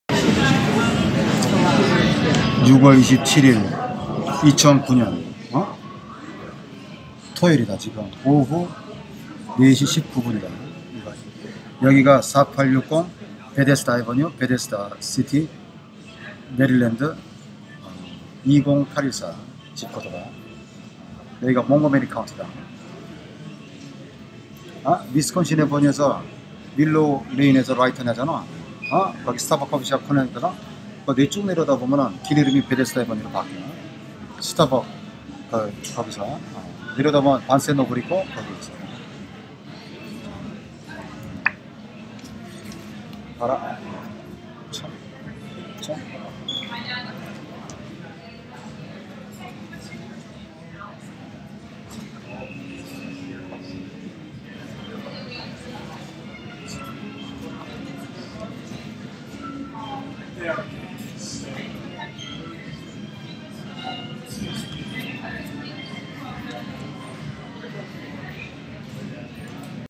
Movie 1
The map in youtube is before construction of this Mall and Apartment.
This Apple Computer store is just across from bluemercury store over bethesda ave in bethesda in Maryland.

Jun-27-2009 Saturday pm 3:18
with displayed Apple MacBook Air in
Apple Computer store on
4860 Bethesda Ave
Bethesda M.D 20814